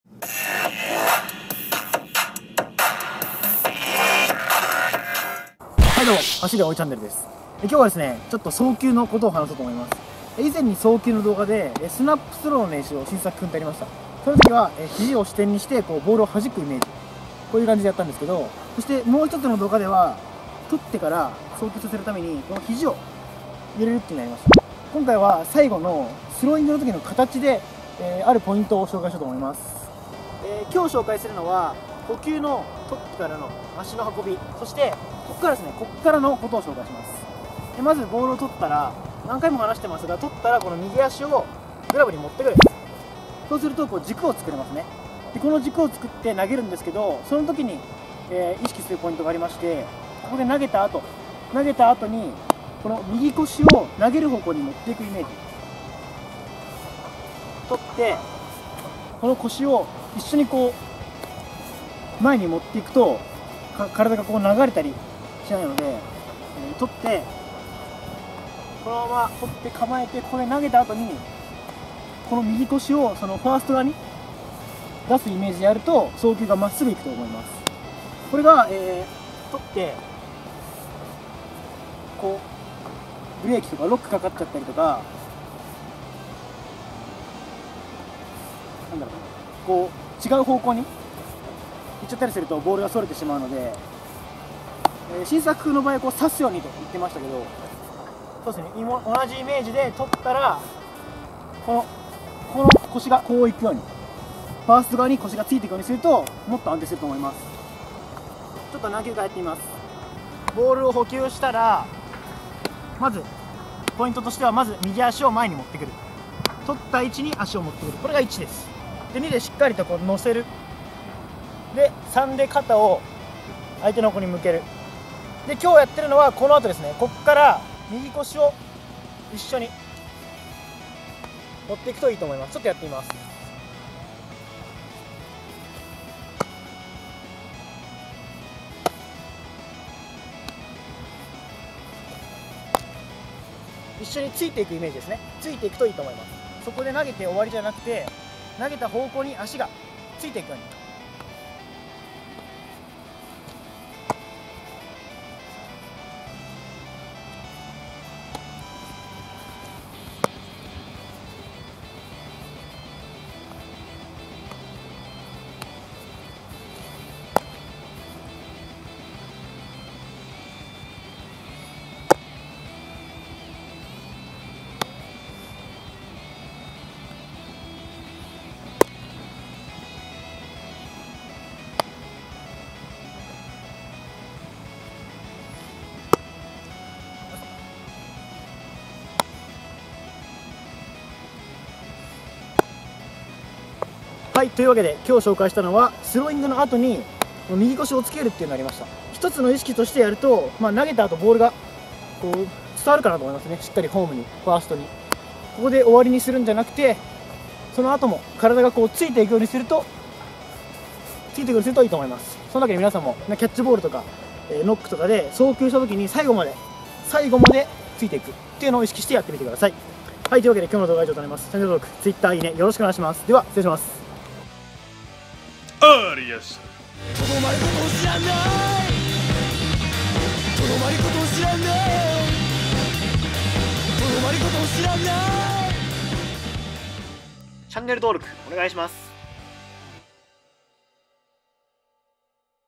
【内野守備】キャッチボールから意識しよう！投げる動作の中で練習から取り入れておきたい１つのポイント！【内野手】【送球の極意】
エンディングや動画内で使われている３４のCDの購入はこちらから！

YSF飛鳥さんLINE ID:a894

YSFはじめさんLINE ID:0m6m

こんにちは。

BGMに使用されている曲